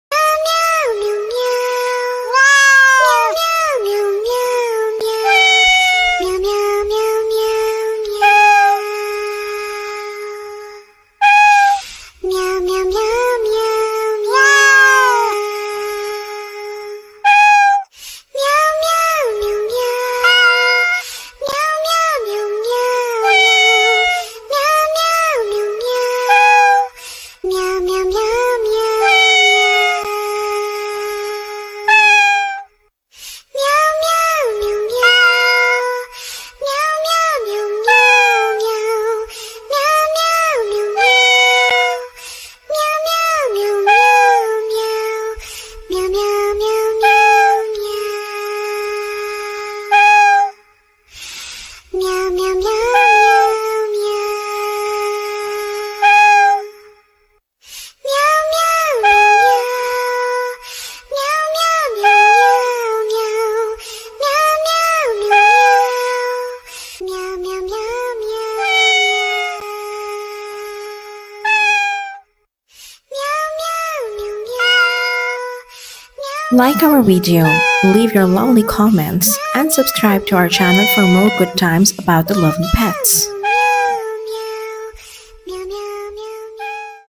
Kitten in Danger: Zombie Attack, Truck to the Rescue! 🧟‍♂️🐱
A brave kitten finds itself in danger as zombies approach! Just when all seems lost, a quick-thinking Truck comes to the rescue. Watch this thrilling adventure unfold as the kitten narrowly escapes a terrifying fate! 🧟‍♂️💥🐱